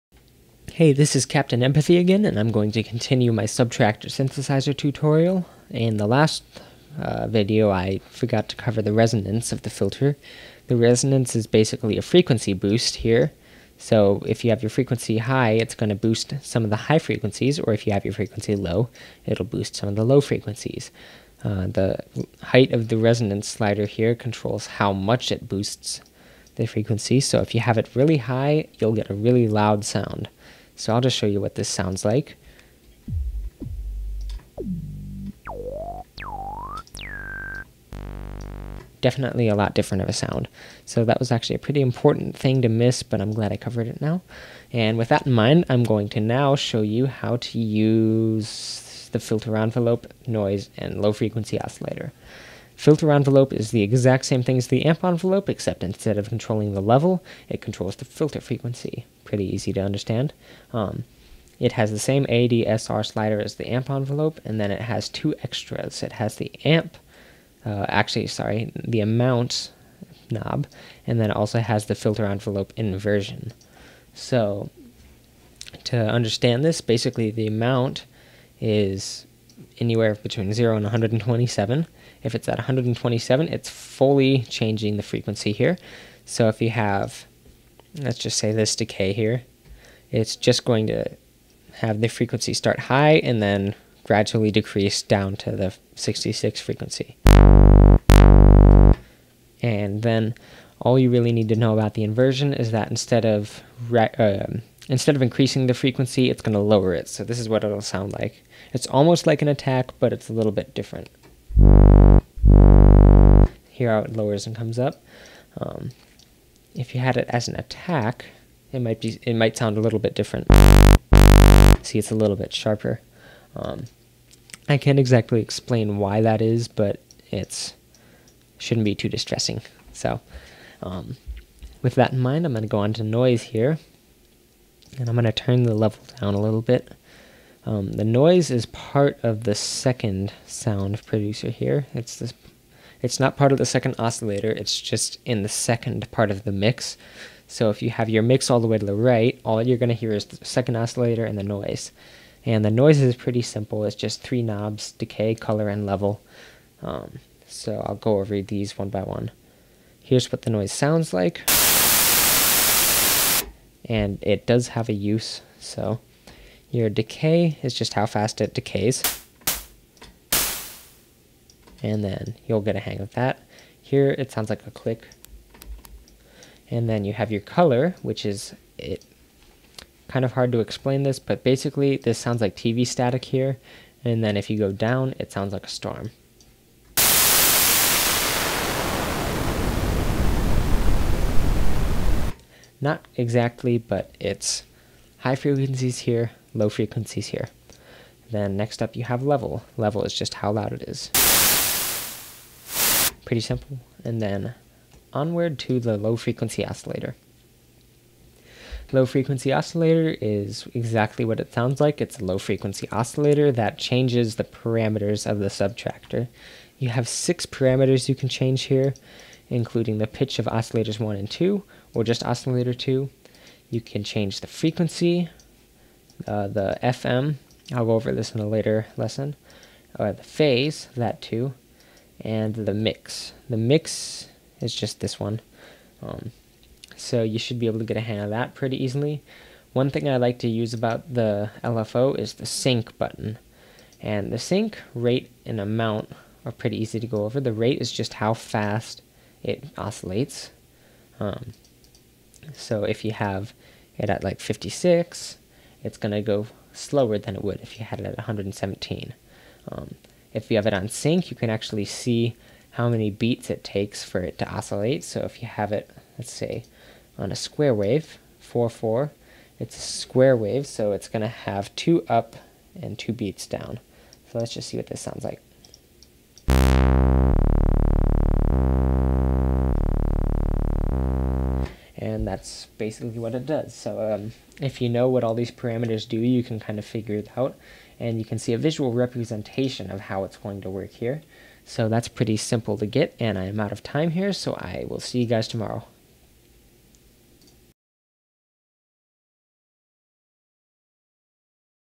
Subtractor Tutorial P2
Warning:
Be careful when watching with headphones. Some of the sounds, like the filter resonance and the noise, can be pretty loud. :-D
In this video, I go over:
 - Filter resonance
 - The filter envelope
 - How to use the noise
 - How to use the low frequency oscillator #1